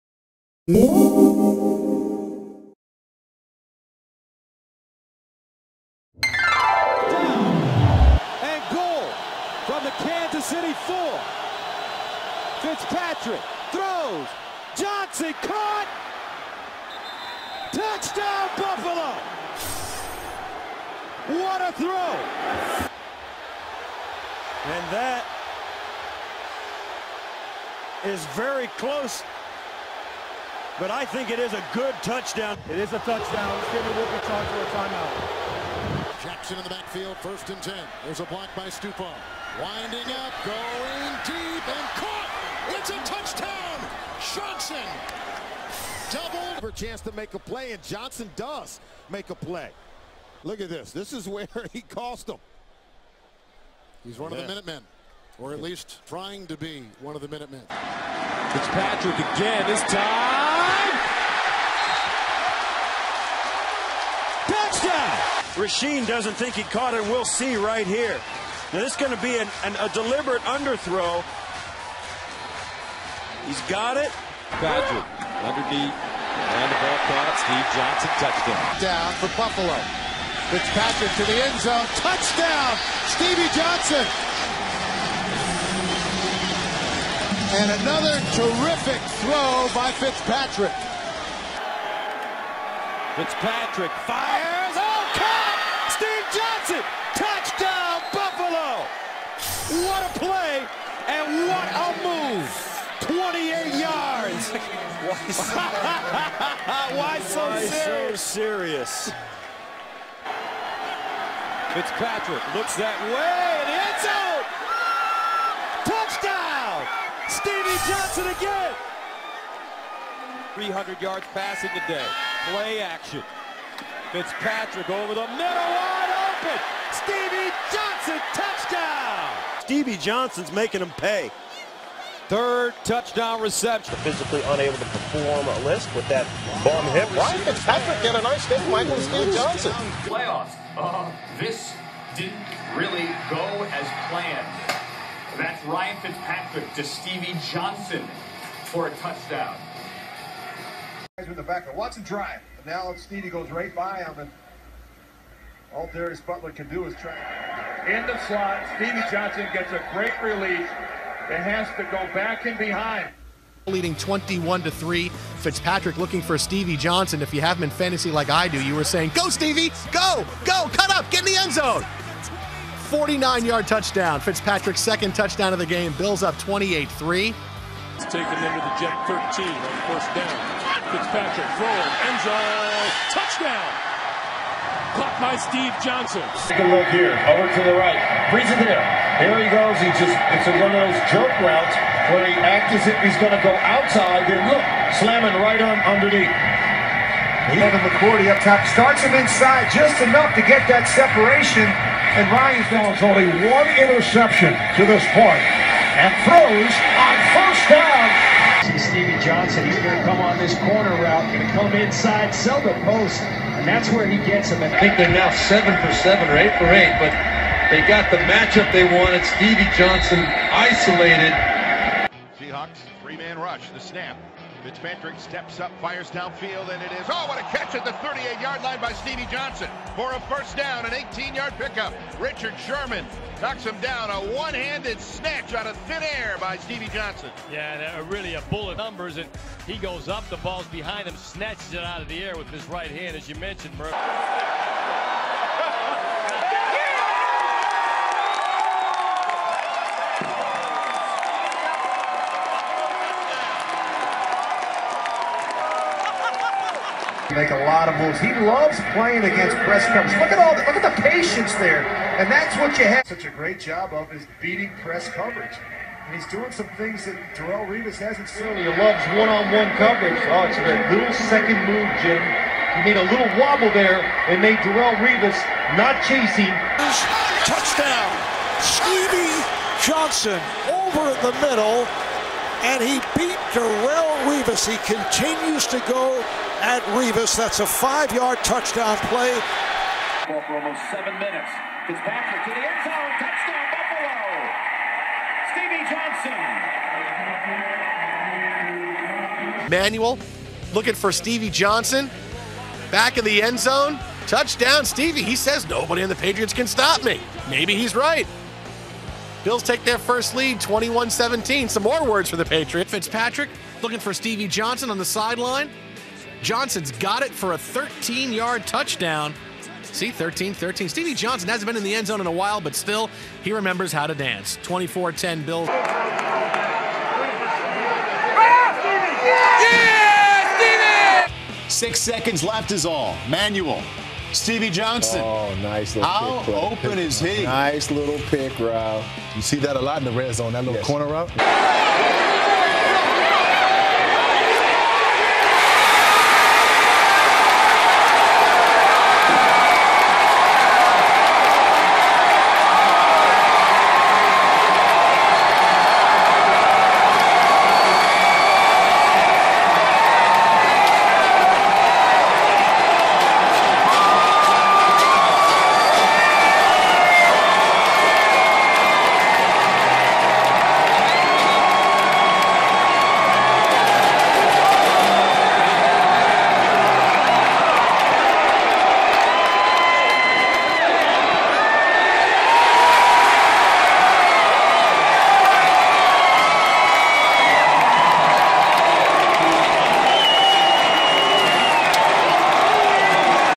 Stevie Johnson Buffalo Bills Career Highlights | NFL Highlights
Stevie Johnson Buffalo Bills Career Highlights. Stevie is without a doubt one of the most colorful Buffalo Bills Wide Receivers. He has had his highs and lows as a buffalo Bills including having three straight 1,000 yard seasons. I personally think of him as one of my top 10 Buffalo Bills of all time. Do you agree?